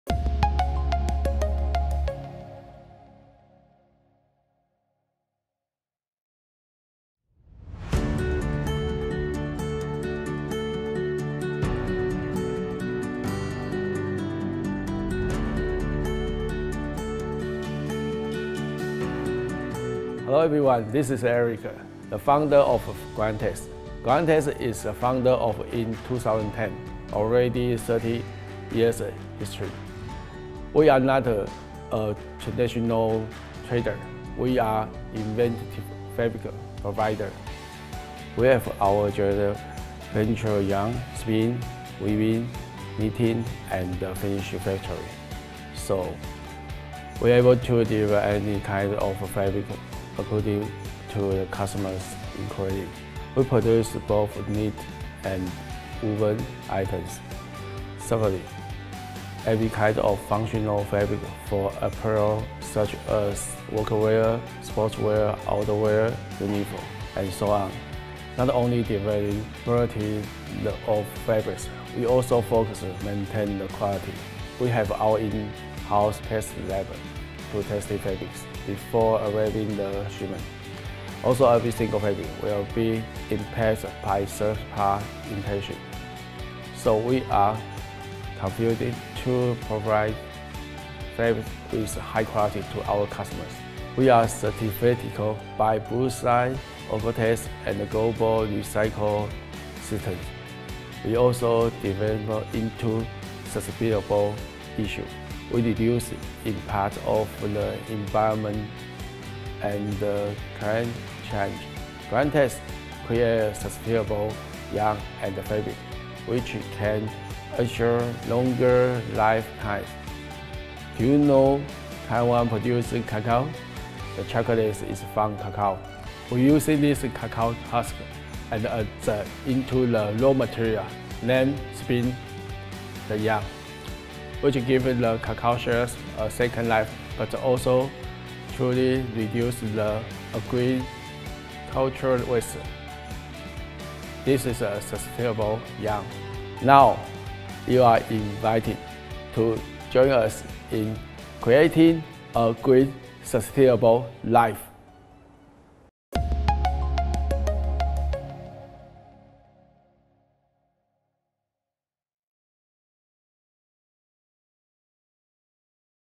GrandeTex Development Co , Ltd
GrandeTex specializes in functional woven and knit fabrics for indoor, outdoor, sportswear and workwear, which mainly export to Europe, Asia and the Pacific area. With a strong management team and textile experts, they offer exceptional quality of textile to the world. Their in-house experts control materials quality and stability. GrandeTex focuses on deeper current client new development, and keeps client product range wider and more flexible.